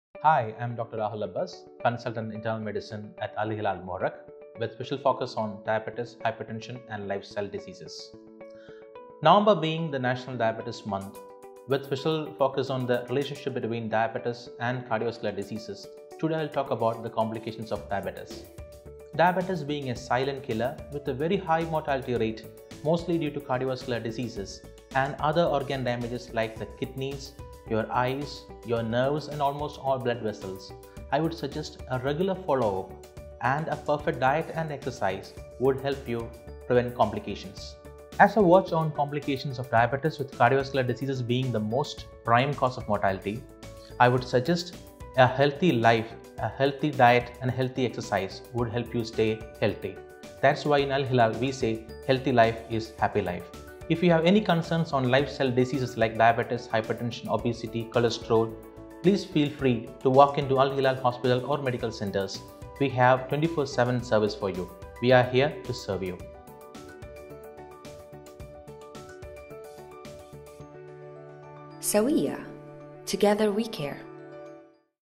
Let's Together Defeat Diabetes.
On this World Diabetes Month Check what Dr. Rahul Abbas Consultant Internal Medicine working at Al Hilal Muharraq expertise in life style diseases talks about diabetes and how to keep check on it. Let's Together Defeat Diabetes.